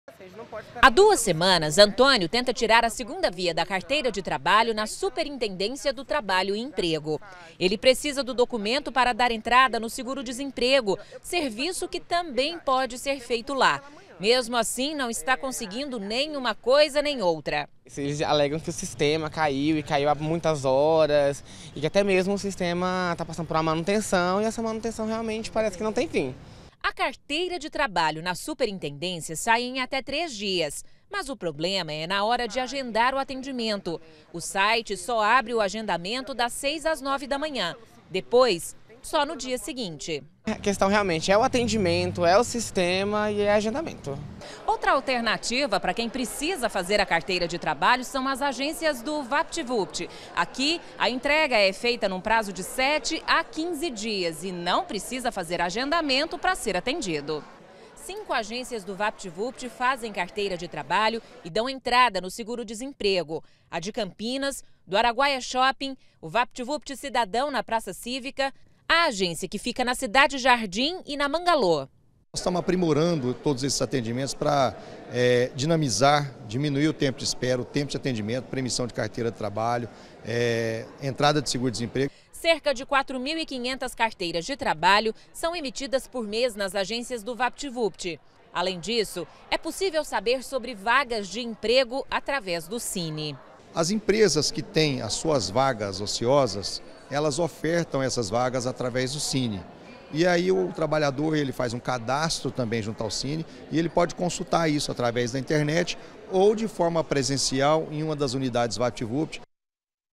CARTEIRA DE TRABALHO: DOCUMENTO TAMBÉM É FEITO EM VAPT VUPT
E não está fácil tirar a carteira de trabalho, aqui em Goiânia. Quem precisa, diz que têm dificuldade pra agendar o atendimento na superintendência do trabalho. E, quando consegue, o sistema cai.